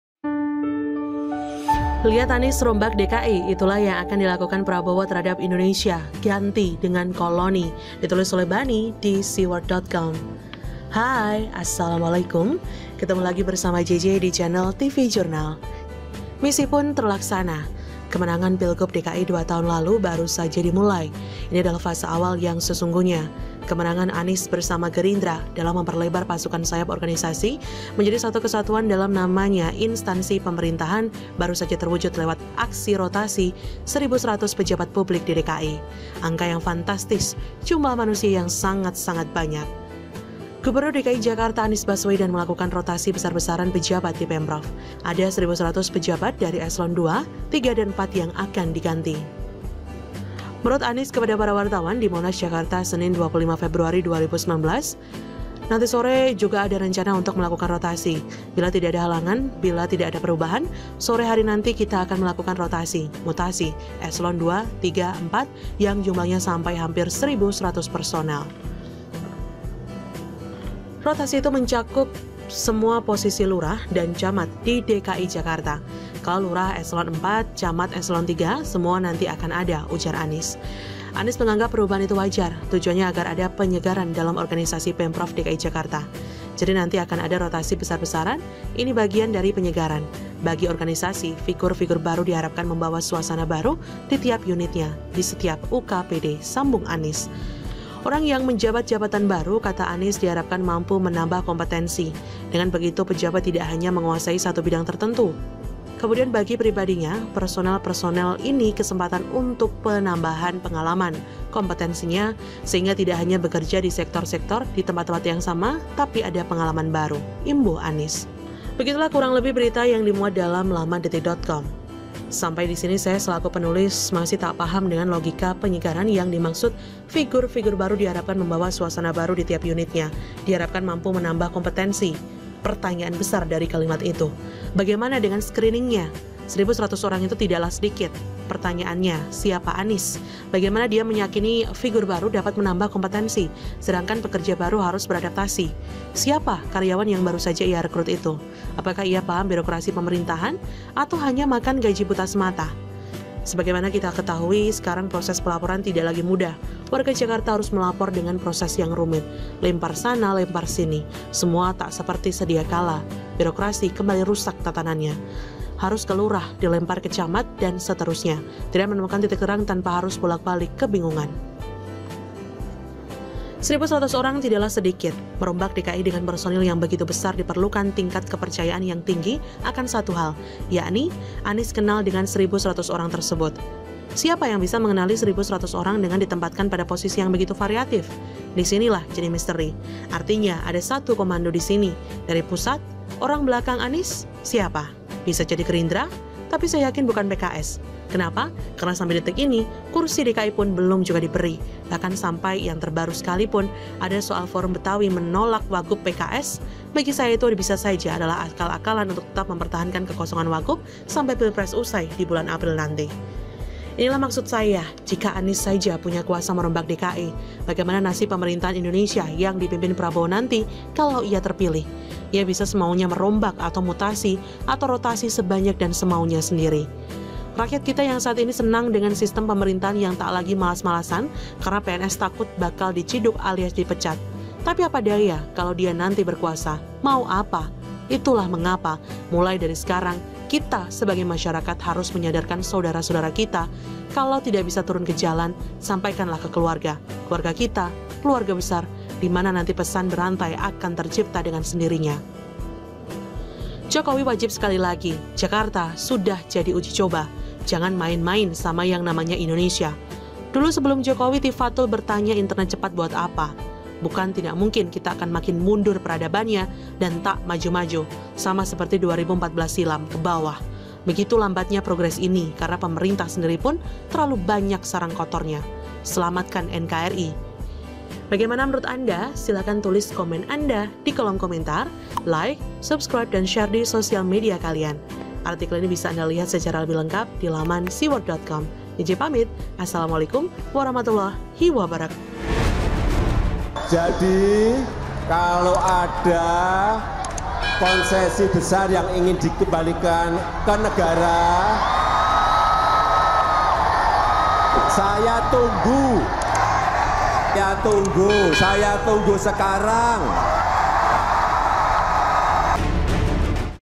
Ganti Koloni ! Anies Rombak DKI, Itulah yang Akan Dilakukan Prabowo Terhadap Indonesia ?
Lihat Anies Rombak DKI, Itulah yang Akan Dilakukan Prabowo Terhadap Indonesia. Ganti Dengan Koloni.